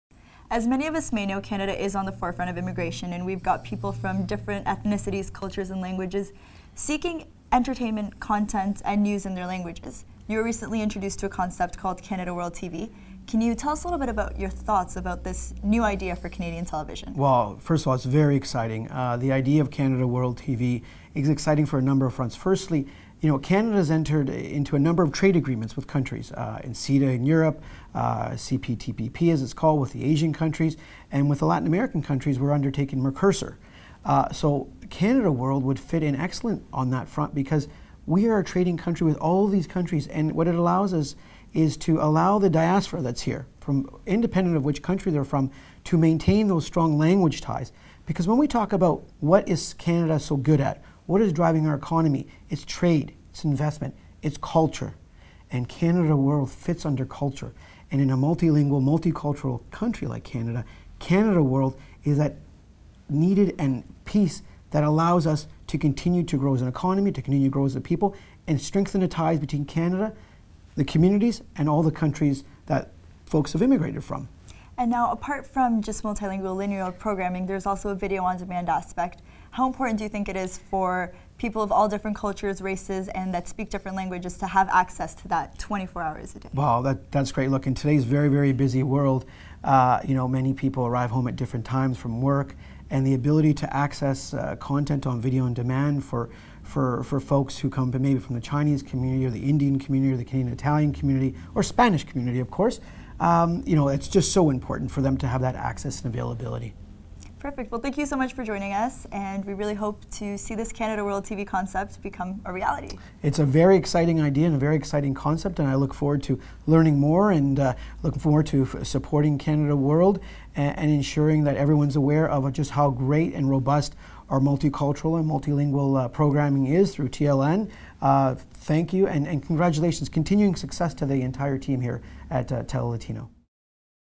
MP Francesco Sorbara Talks About CanadaWorld TV
Francesco Sorbara, MP for Vaughan-Woodbridge speaks with reporter Camila Gonzalez about our new CanadaWorld TV channel initiative.